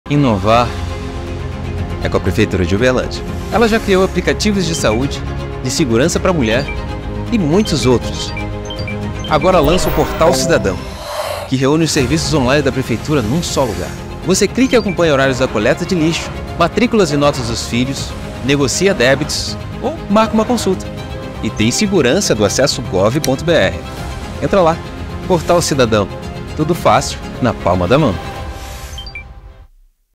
Portal Cidadão: tudo fácil, na palma da mão!
Prodaub | A Prefeitura de Uberlândia acaba de lançar uma iniciativa inovadora, o Portal Cidadão. O sistema reúne os serviços municipais em um só lugar. É possível realizar agendamentos, negociar débitos, consultar horários da coleta de lixo, entre outros. Acesse e confira